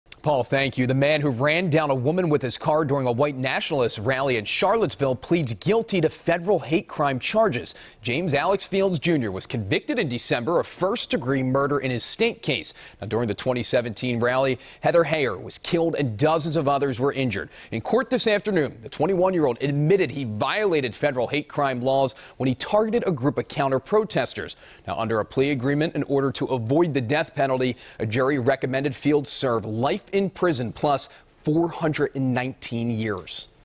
Charlottesville Attacker Could Change Plea After Conviction
James Alex Fields Jr., of Maumee, Ohio, was convicted in state court in December in the death of anti-racism activist Heather Heyer and for injuring dozens more.